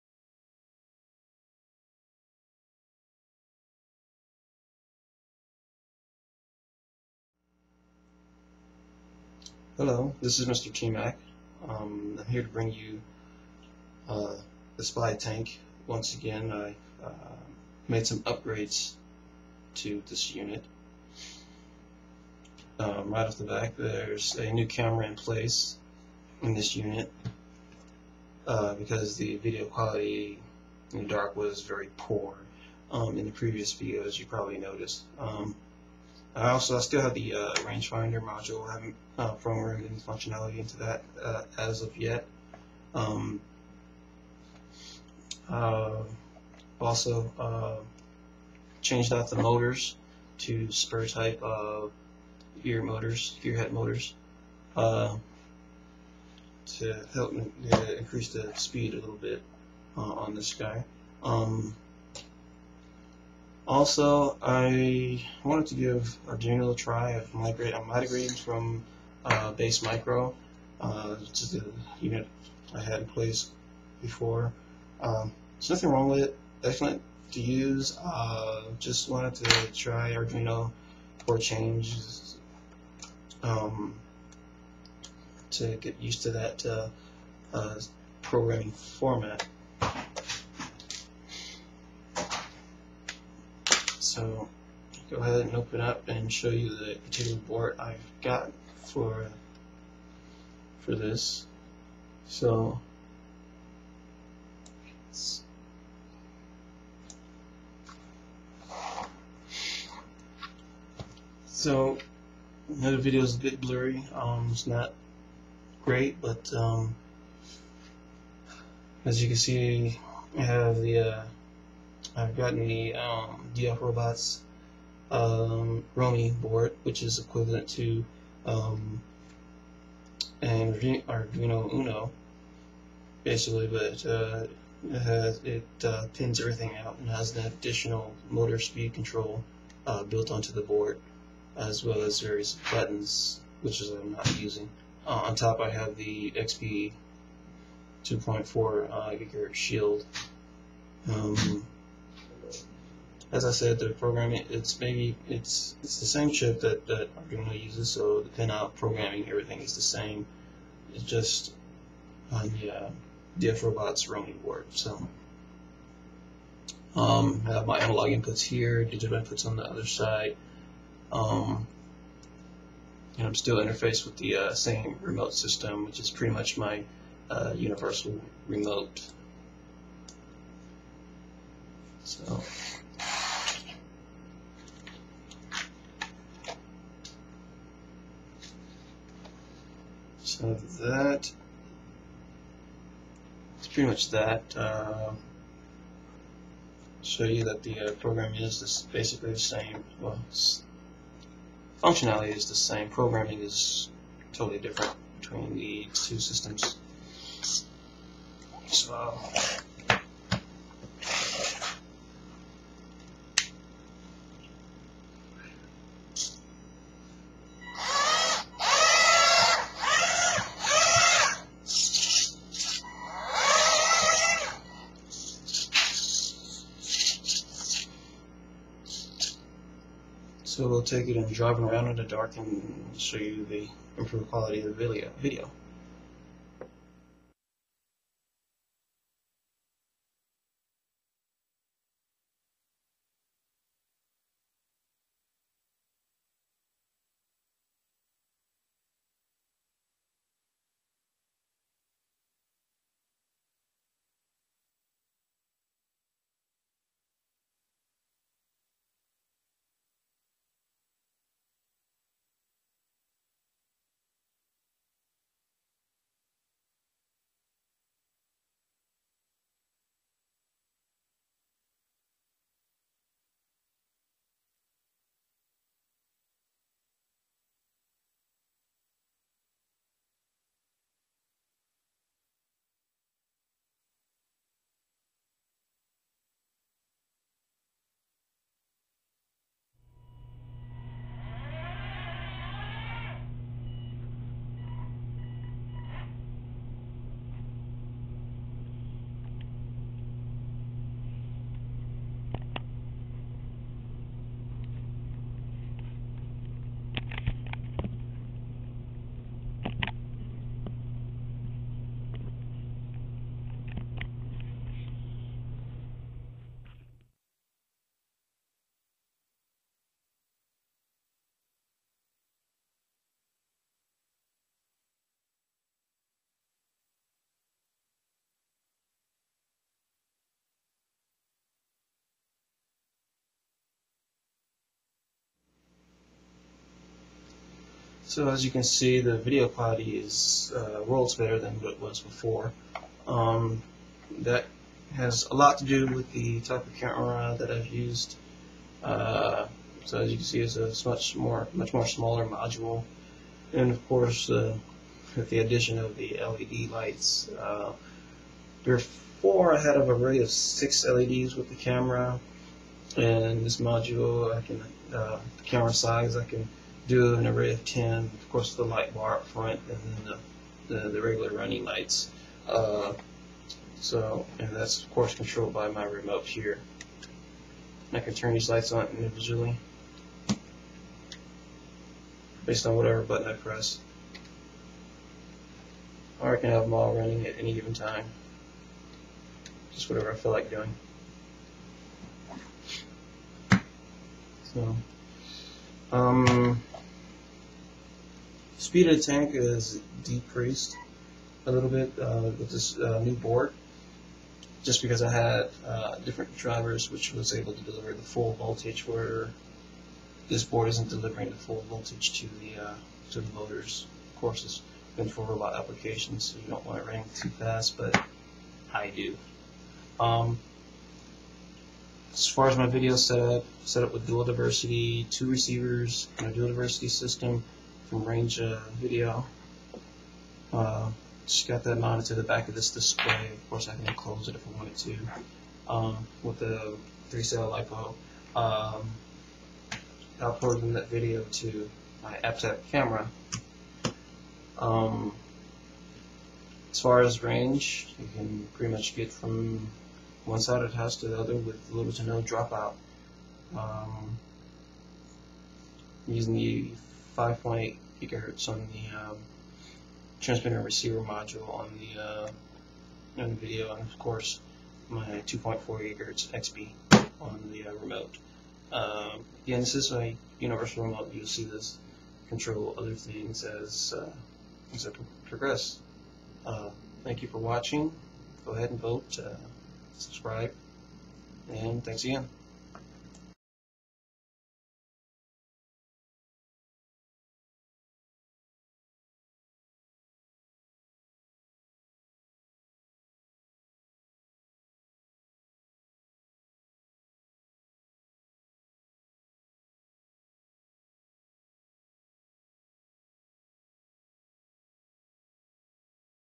Mr. Tmac's RC Spy tank upgrades
made some improvements to this project. new motors, new camera, and MORE leds. i am also giving Adruino code a shot. using the DF robots Romeo-All in one Controller with 2A 2ch h-bridge.